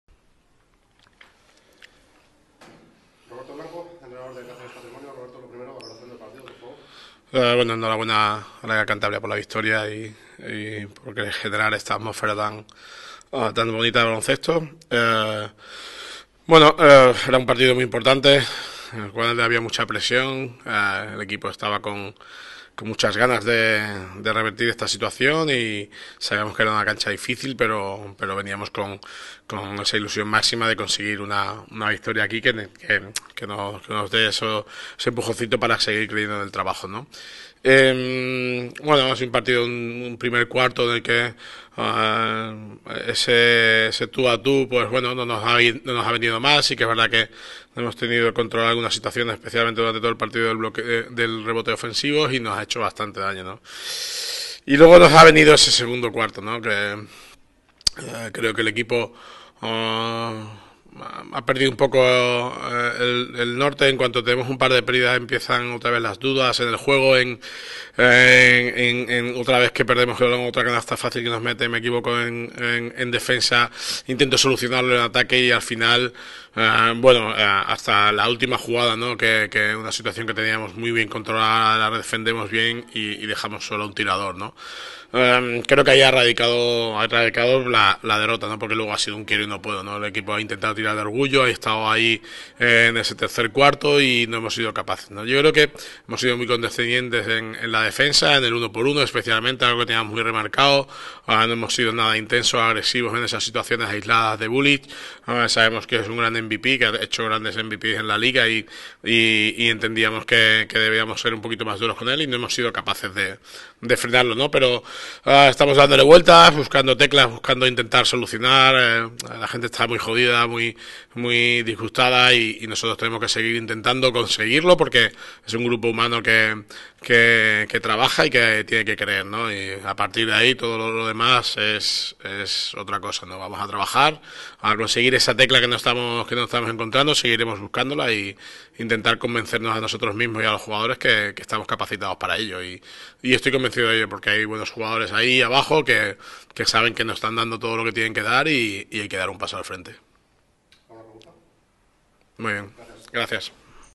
T23/24 RP Roberto Blanco tras el partido en Cantabria
Rueda de prensa del entrenador el Cáceres Patrimonio de la Humanidad tras el partido en Cantabria.
(Video GAC)